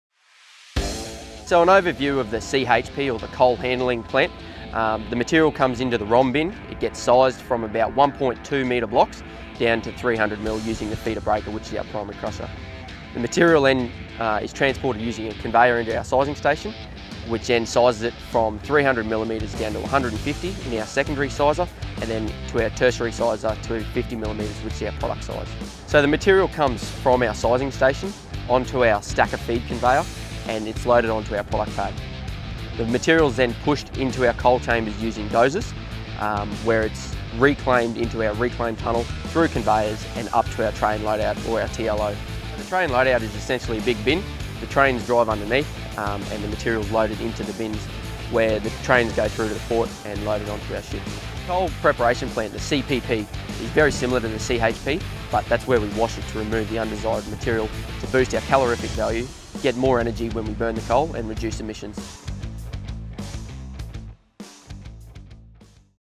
Get to know our CHPP | CHPP Project Engineer Giulian talks about the CHPP at the Carmichael Mine
This one is for those fans who have been asking for more info about our CHPP.

We've put together this video of our CHPP Project Engineer Giulian giving a brief overview of the CHPP (Coal Handing and Preparation Plant) project at the Carmichael Mine.

You may remember Giulian as one of the faces from our first graduate program! Giulian joined us as a gradate engineer over two years ago and is now our CHPP Project Engineer.